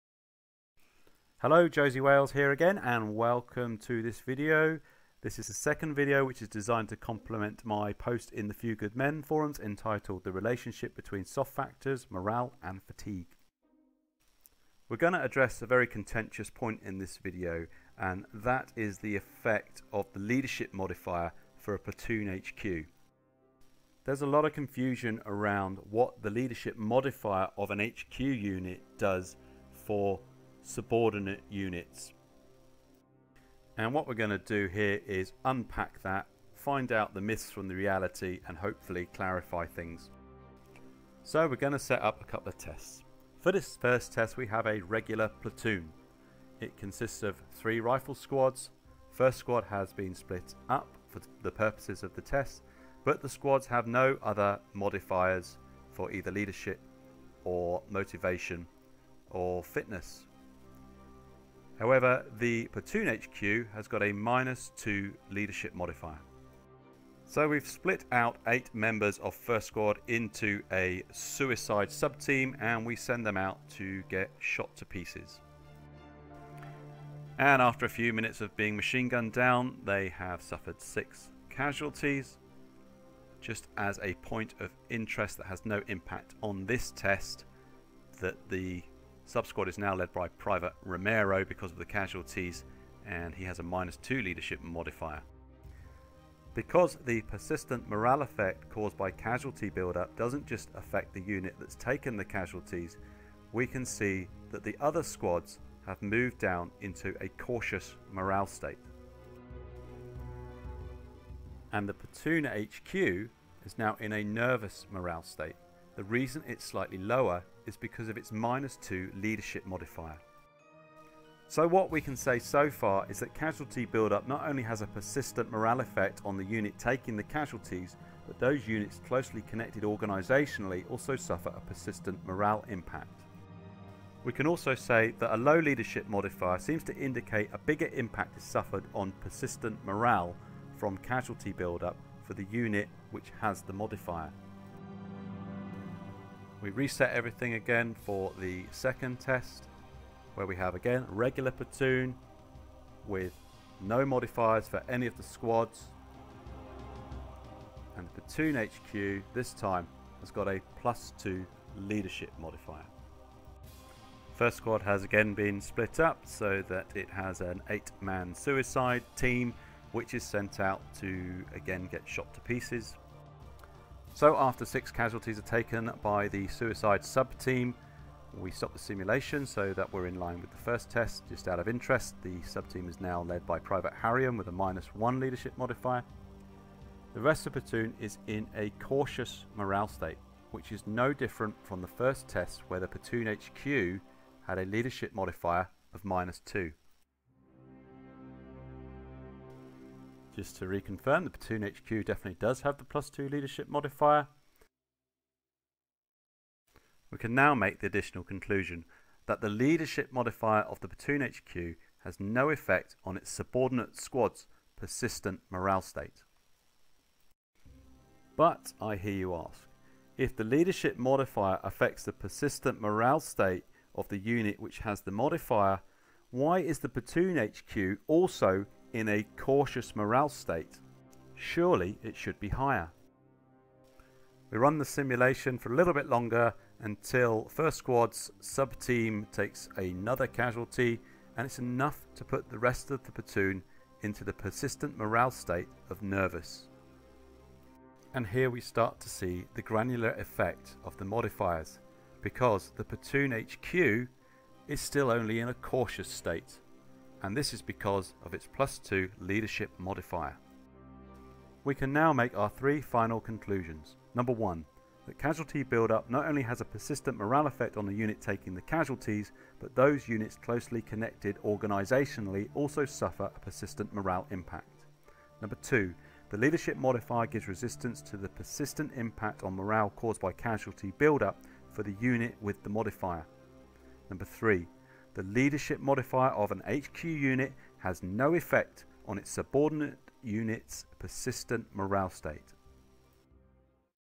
HQ Leadership Modifier
The 2nd complimentary video to my post on the Few Good Men forums entitled 'The Relationship between Soft Factors, Morale & Fatigue'

Music;